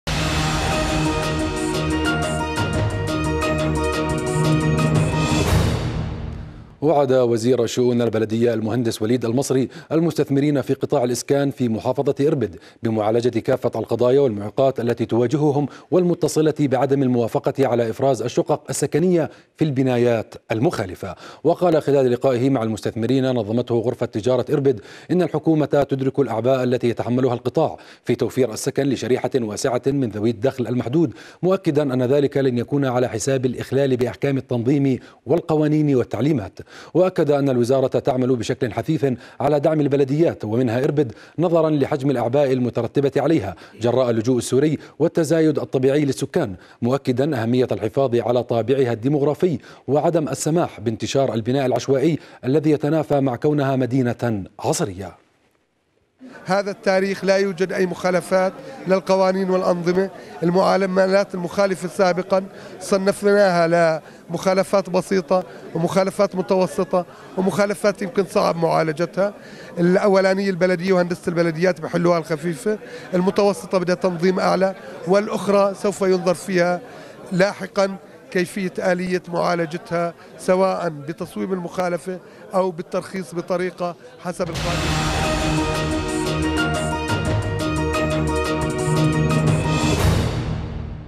وزير البلديات يعد المستثمرين بقطاع الاسكان في إربد بحل مشاكلهم
وعد وزير الشؤون البلدية المهندس وليد المصري المستثمرين في قطاع الاسكان في محافظة اربد بمعالجة كافة القضايا والمعوقات التي تواجههم، والمتصلة بعدم الموافقة على افراز الشقق السكنية في البنايات المخالفة.

 وقال خلال لقائه مع المستثمرين نظمته غرفة تجارة اربد ان الحكومة تدرك الاعباء التي يتحملها القطاع في توفير السكن لشريحة واسعة من ذوي الدخل المحدود، مؤكدا ان ذلك لن يكون على حساب الاخلال باحكام التنظيم والقوانين والتعليمات.